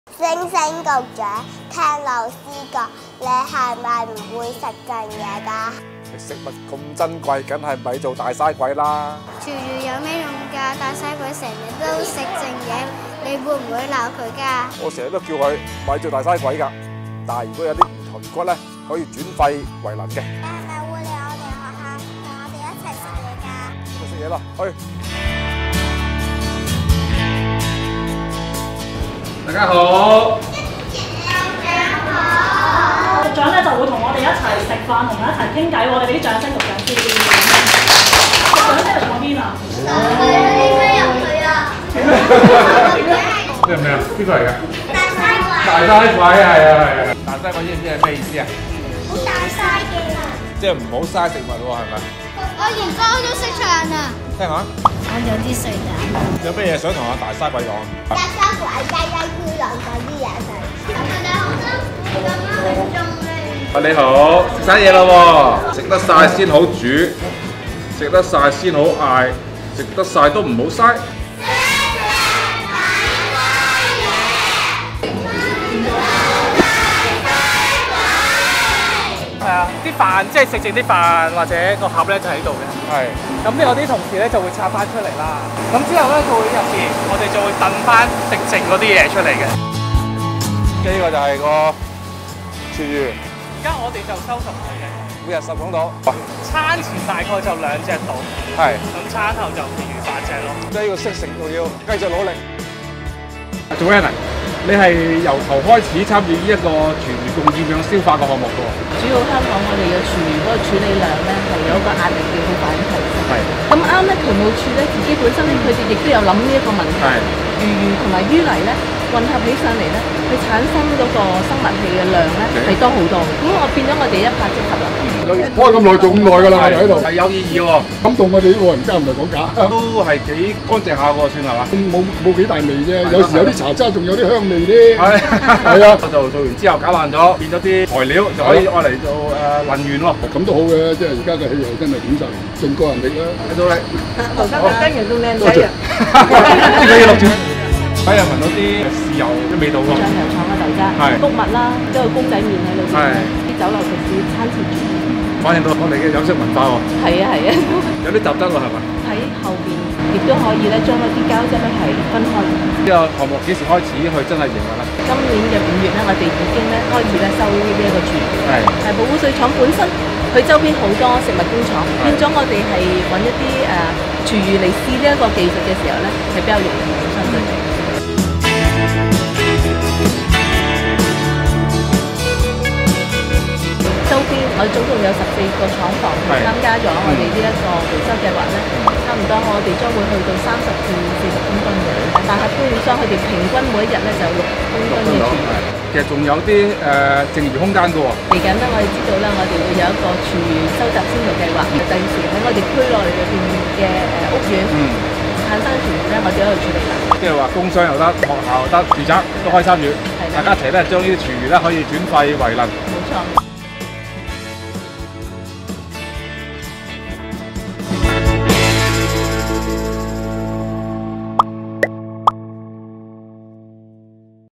廚餘回收 (二)： 中央處理 創新「消化」
幾個月前，網誌題為「廚餘回收：中央處理 發電為本」，簡介了香港廚餘回收的新里程及大圖像，一方面減堆填區壓力，另一方面添本地可再生能源，一舉兩得！

「大哥加二哥」廚餘消化多

上回介紹北大嶼O·PARK1，此先進設施在本港有機資源回收中心網絡中，堪稱「大哥」！去年中率先出世，善用厭氧分解 (Anaerobic Digestion) 技術，轉廚餘為生物氣，產熱產電。

上文亦透露了「二哥」的行踪！為加快加大本地轉廚餘為能源的整體消化量，除覓地建專廠，北大嶼O·PARK1及北區O·PARK2外，政府靈活變通，創新研究善用合適污水處理廠及其邊陲空間，開展「廚餘及污泥共厭氧消化」 (Anaerobic Co-digestion)。近日，我走訪大埔污水處理廠廚餘回收先導項目，簡稱「共厭氧消化」吧！